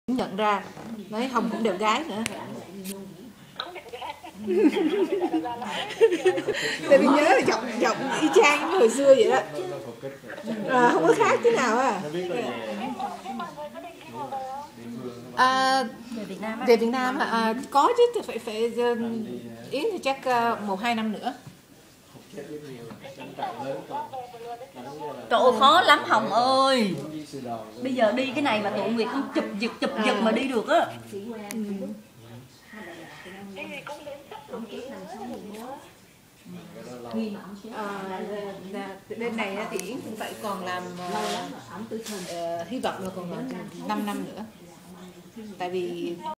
Từ Seattle goi phone cho NhaTrang
Phi Yến, Nguyệt và Mạnh Tiến gọi phone cho Nha Trang...